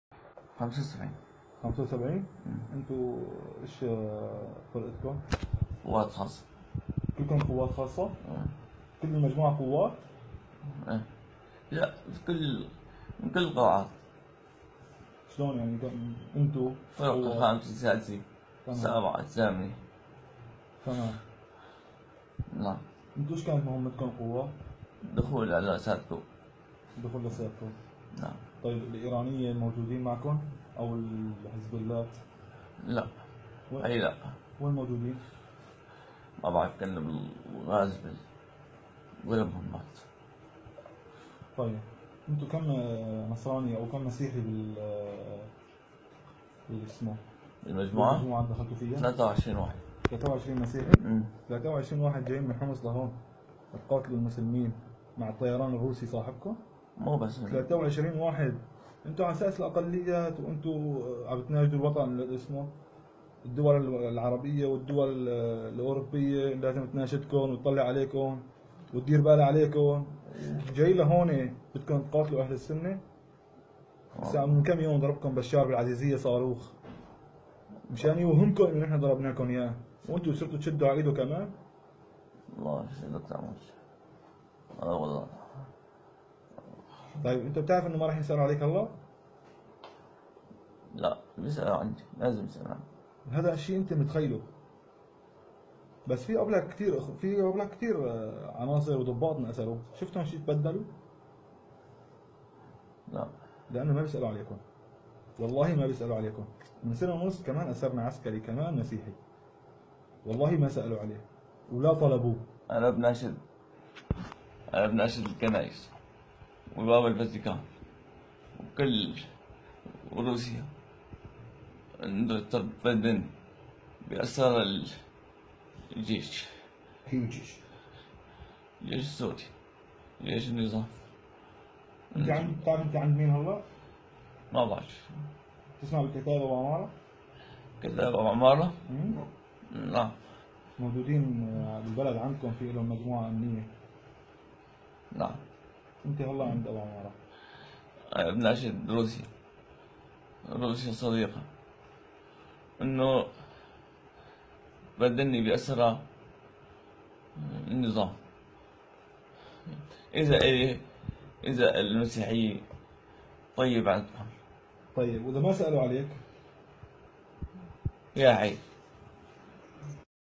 ملازم في جيش الأسد يناشد روسيا وبوتين لمبادلته
+18
CAUTION! - this video contains footages which might NOT be suitable for all audience

بث المكتب الإعلامي لـ"كتائب أبو عمارة" تسجيلاً مصوراً فيديو لضابط في جيش النظام  مصاب أسرته أمس كتائب أبو عمارة على جبهة الراموسة في ريف حلب الجنوبي.